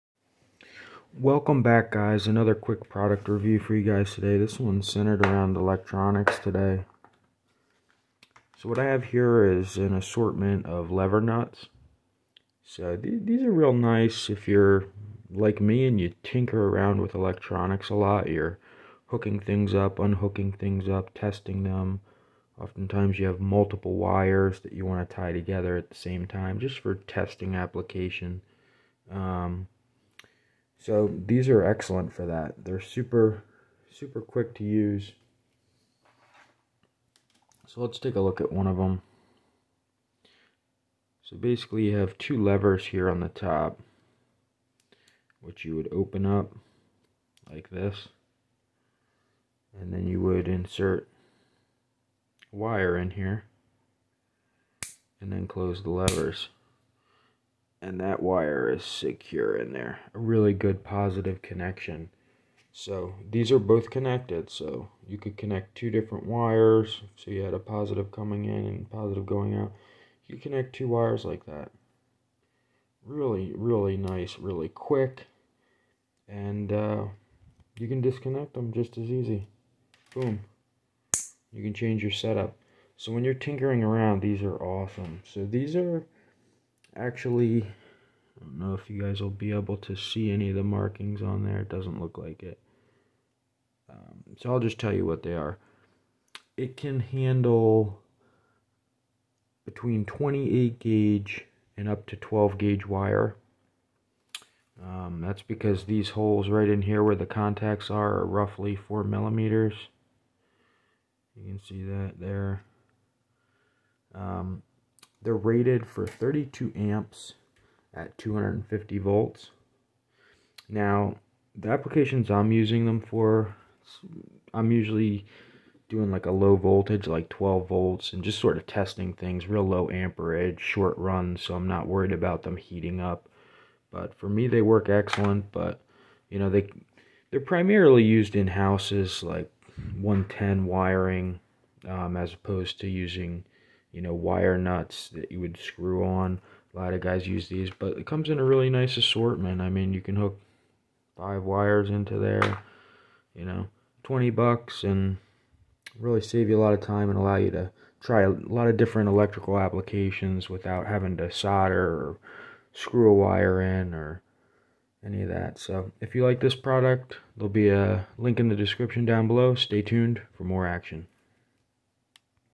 Lever Nuts for 110V and DIY 12V
A quick review of these easy to use lever nuts.  A must have for any DIY electronics tinkerer.